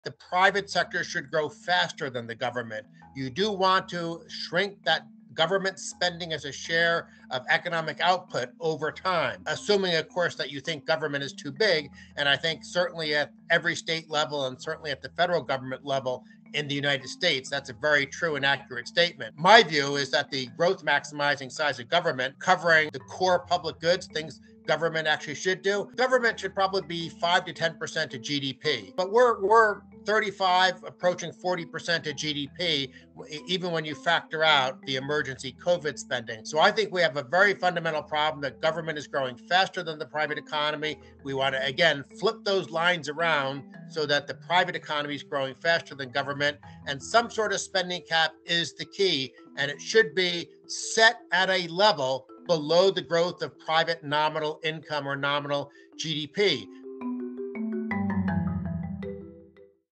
Why the GOVERNMENT Should NOT Grow Faster Than the PRIVATE SECTOR
to hear the full conversation with: Dan Mitchell from the Center for Freedom and Prosperity, and Barry Poulson Professor of Economics at the University of Colorado.